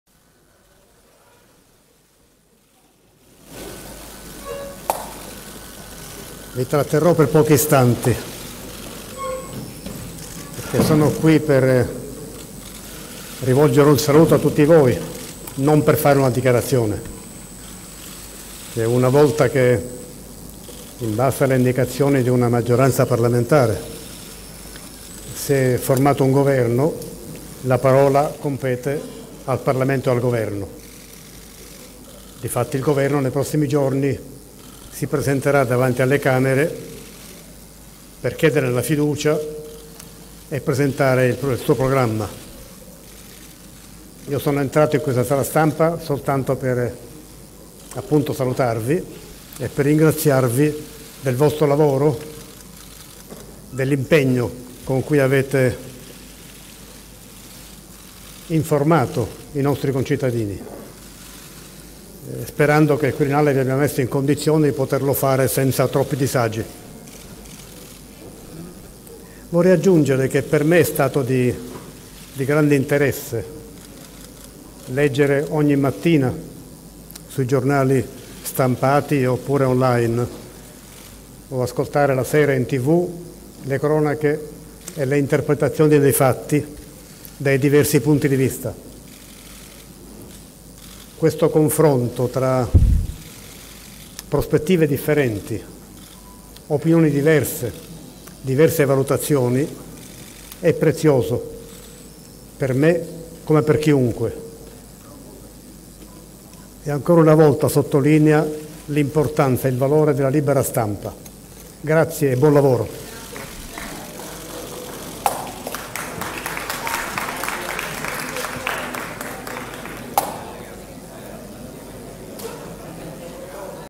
Mattarella: Si è formato nuovo governo ora la parola passa al Parlamento"
"E' stato molto interessante leggere in questi giorni i giornali"

POLITICA (Roma). Mattarella: "Si è formato un governo, la parola ora compete al Parlamento e al governo che nei prossimi giorni si presenterà alle Camere per chiedere la fiducia e presentare il suo programma". Lo dice il presidente della Repubblica Sergio Mattarella, dopo che Giuseppe Conte ha presentato la lista dei ministri. (Giuliano Rosciarelli/alanews)